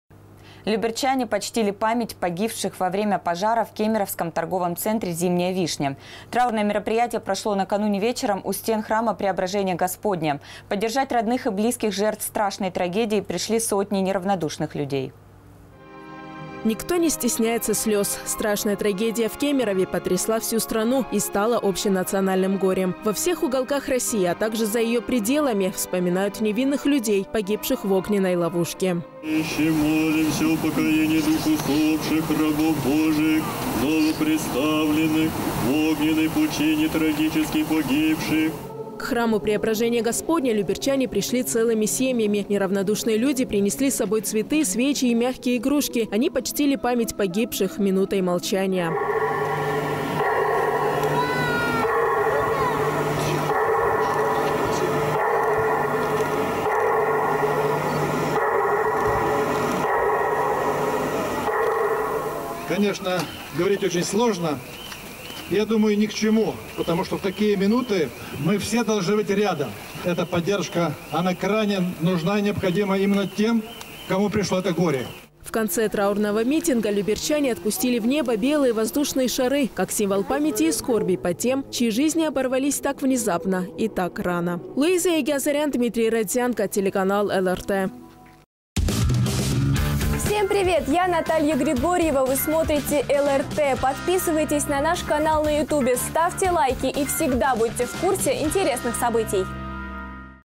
Люберчане почтили память жертв кемеровской трагедии
Люберчане почтили память погибших во время пожара в кемеровском торговом центре. Траурное мероприятие прошло накануне вечером у стен Храма Преображения Господня. Поддержать родных и близких жертв страшной трагедии пришли сотни неравнодушных людей.

Никто не стесняется слез. Страшная трагедия в Кемерове потрясла всю страну и стала общенациональным горем. Во всех уголках России, а также за ее пределами вспоминают невинных людей, погибших в огненной ловушке.

К Храму Преображения Господня люберчане пришли целыми семьями. Неравнодушные люди принесли с собой цветы, свечи и мягкие игрушки. Они почтили память погибших минутой молчания.

Владимир Ружицкий, глава городского округа Люберцы:
"Конечно, говорить очень сложно, и, я думаю, ни к чему, потому что в такие минуты мы все должны быть рядом. Эта поддержка крайне нужна и необходима именно тем, кому пришло это горе".

В конце траурного митинга люберчане пустили в небо белые воздушные шары, как символ памяти и скорби по тем, чьи жизни оборвались так внезапно и так рано.